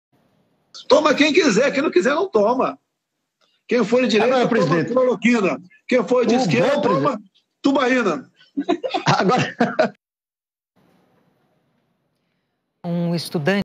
BOLSONERO ZOMBA DO COVID19,RENATA VASCONSELLOS O FUZILA COM OLHAR
O nosso canal  não é de direita, O nosso canal não é de esquerda.O nosso canal é direto”é a favor do Brasil!
BOLSONERO ZOMBA DO COVID19,RENATA VASCONCELOS FUZILA COM OLHAR